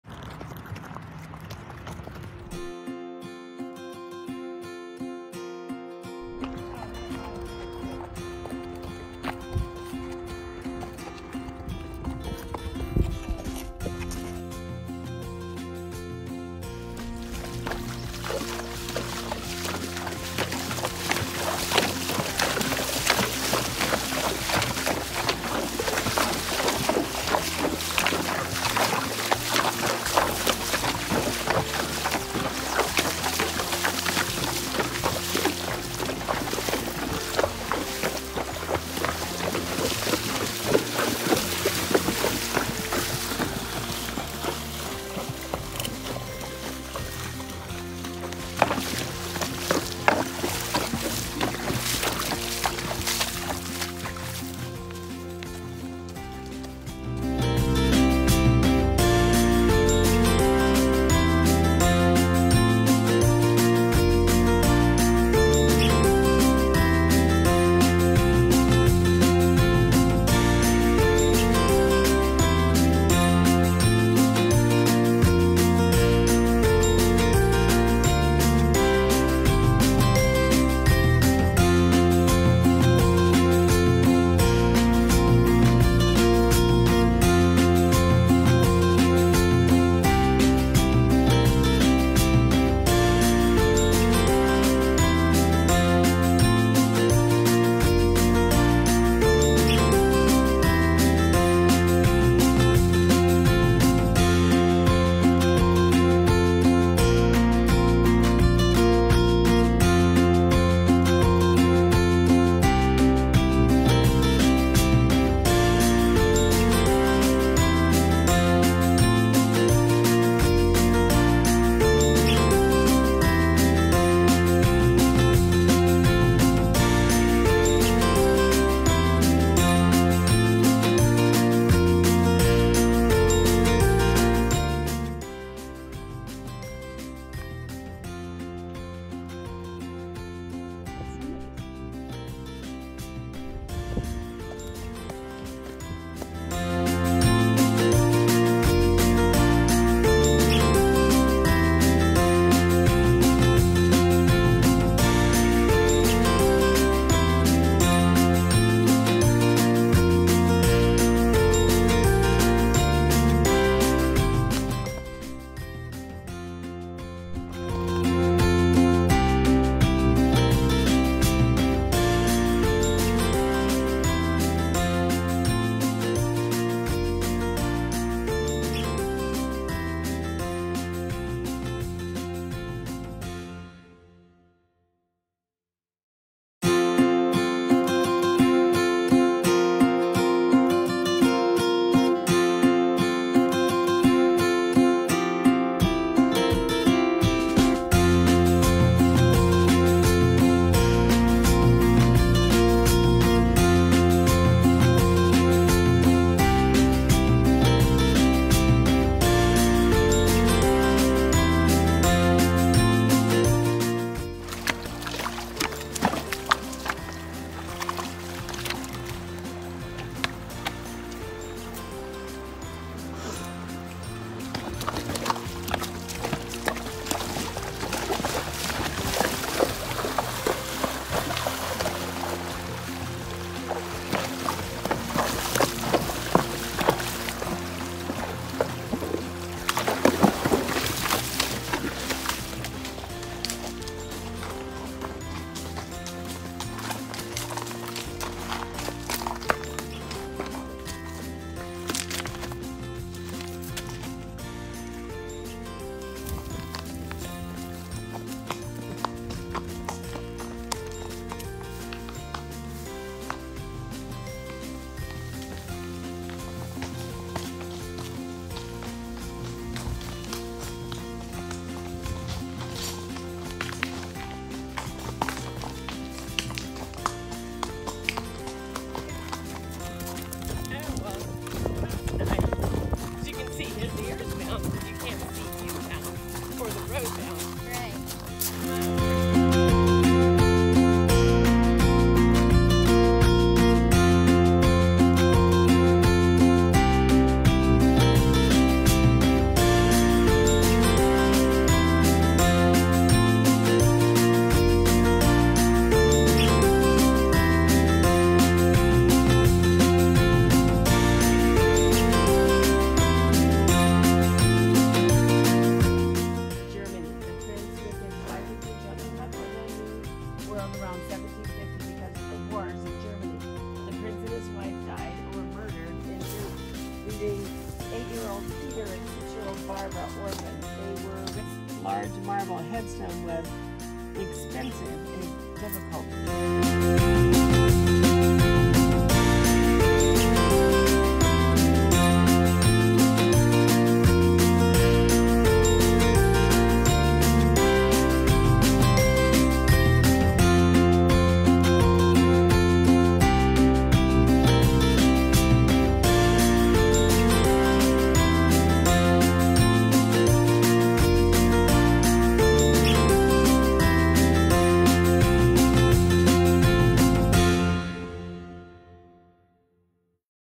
Riding Horses at Los Peñasquitos Canyon Preserve in San Diego, California.
A ride through the streams, pass by the vegetable gardens. A little history of the ranch house cook with tombstone/ gravesite. We saw lots of happy San Diegans  out for a walk, biking or visiting the waterfall, this video shows a glimpse of our ride at Los Peñasquitos